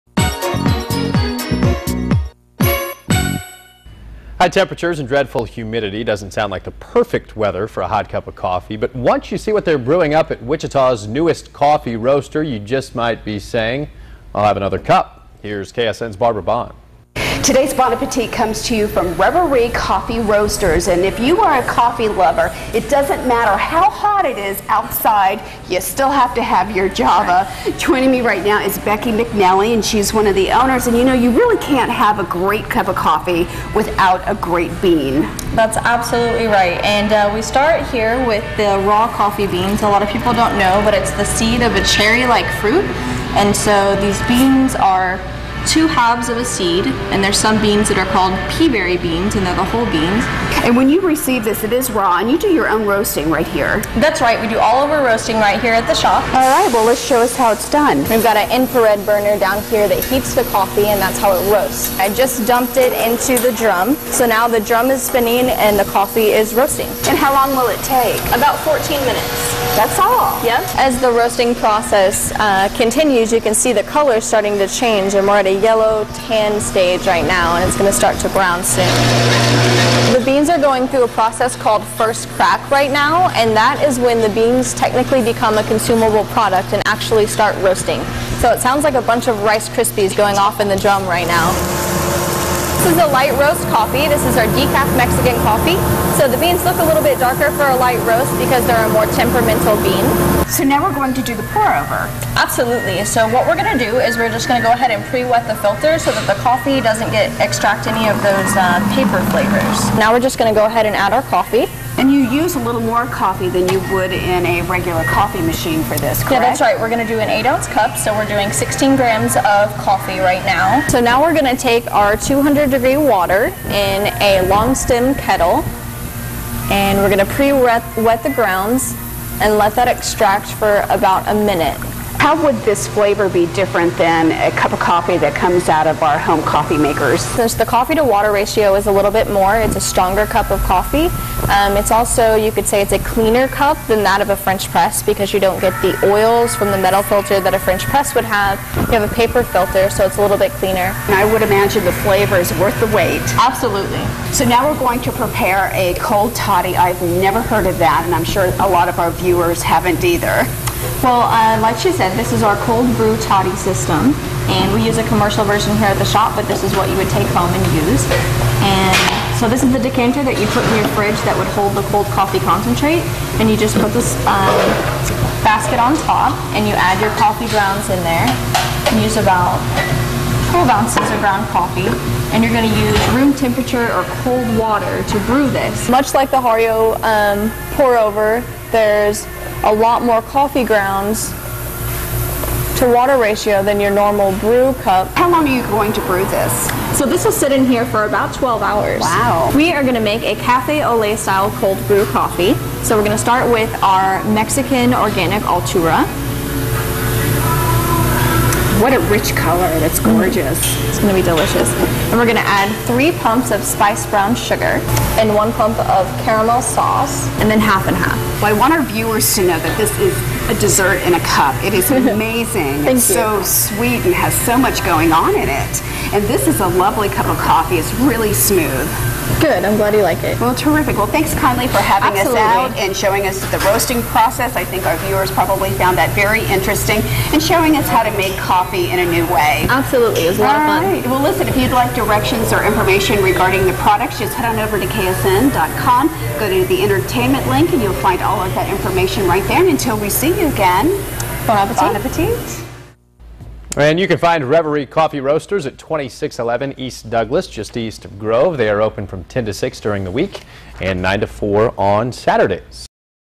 Coffee recipes from Reverie Coffee Roasters
Today's coffee recipes comes from guest roaster, Becky McNelly.